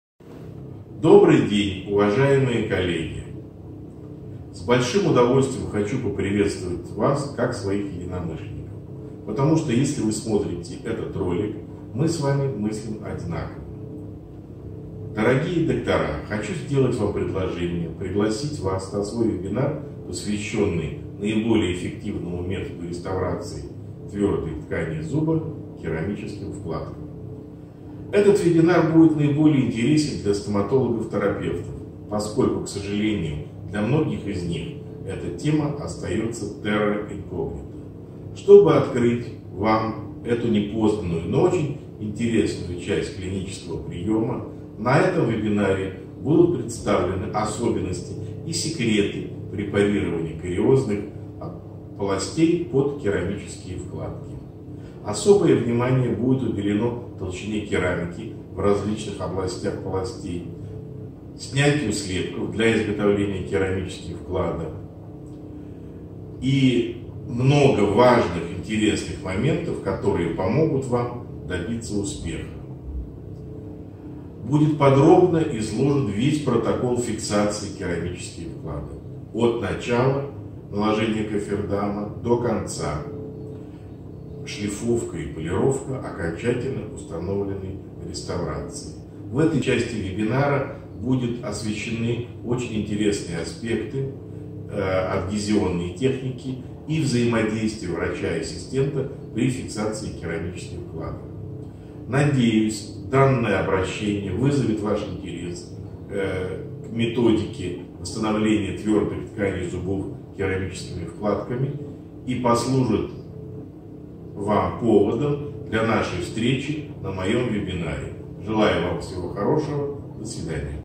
Мощиль А.И._ приглашение на Вебинар по вкладкам
Вебинар "Керамические вкладки как эффективный метод эстетической реставрации твердых тканей зубов. Подробная клиническая инструкция к применению для терапевтов – стоматологов"

Ведущий: Мощиль Андрей Иванович - кандидат медицинских наук, ведущий врач стоматолог-терапевт Клиники "Мегастом АДИ" (г.Москва).
Специализация: все виды терапевтических работ, эндодонтическое лечение, в т.ч. методом вертикальной конденсации, восстановление зубов с использованием керамических вкладок, включая технологию CEREC.

По вопросам участия в вебинаре обращайтесь:
Учебный центр Мегастом.